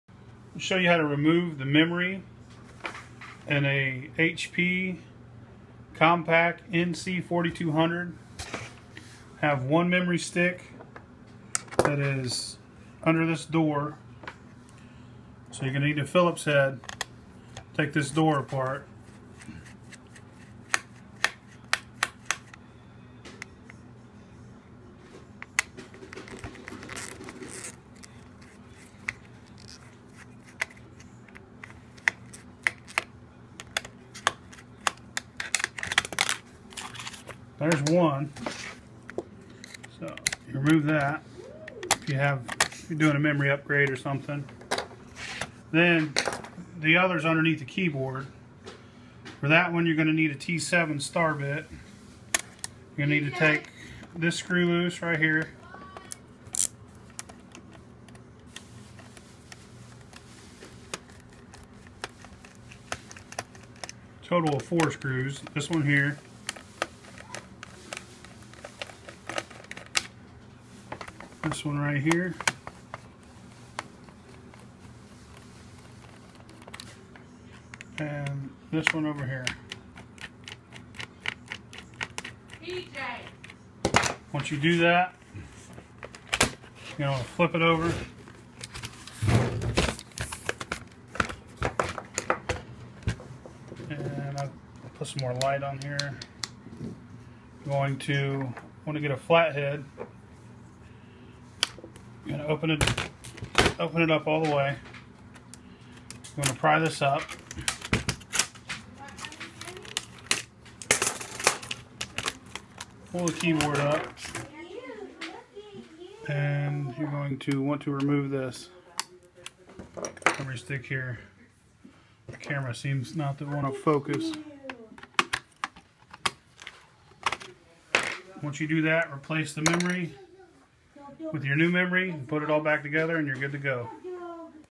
How to Change the Memory in a HP Compaq NC4200 Laptop Computer
Straight to the point instructions on how to install new ram in a HP Compaq NC4200 laptop Computer.